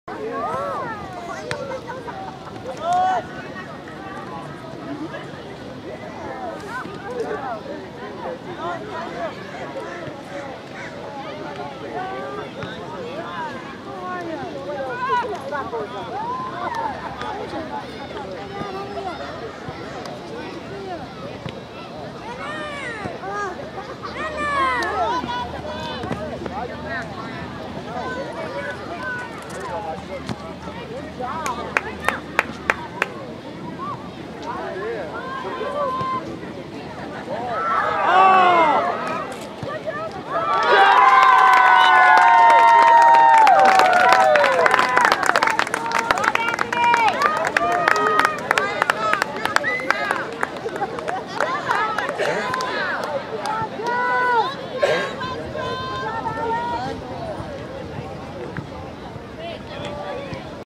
2012 Cerritos Thanksgiving Invitational WGG BU14 All Stars v R75 Sanchez - Trev Scores!
2012 Cerritos Thanksgiving Invitational WGG BU14 All Stars v Region 75 Sanchez 11/24/2012